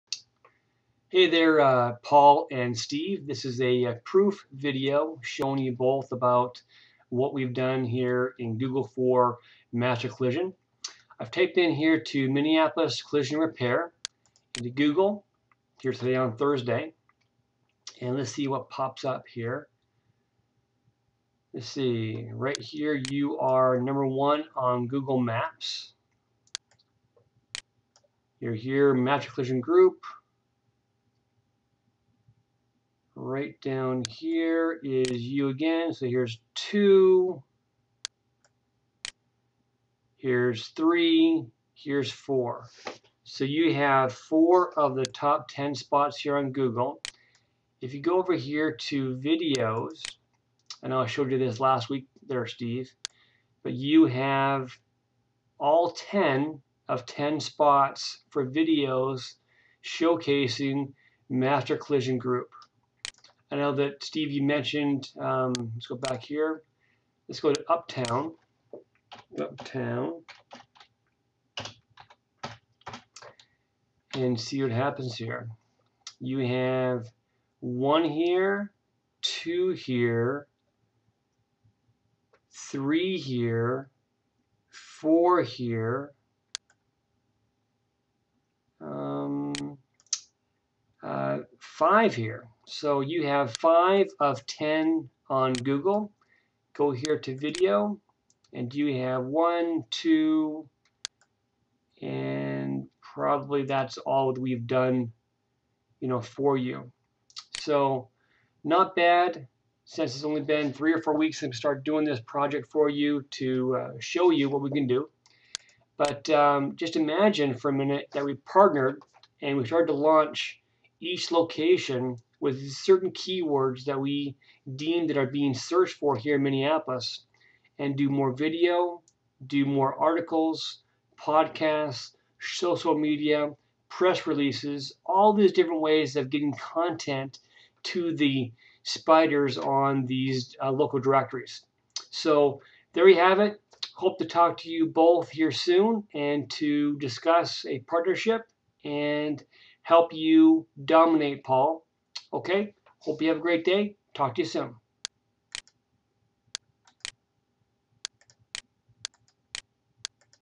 Master Collision, Proof Video, (Minneapolis Collision Repair)
Proof video showing how Master Collision Group is listed on Google.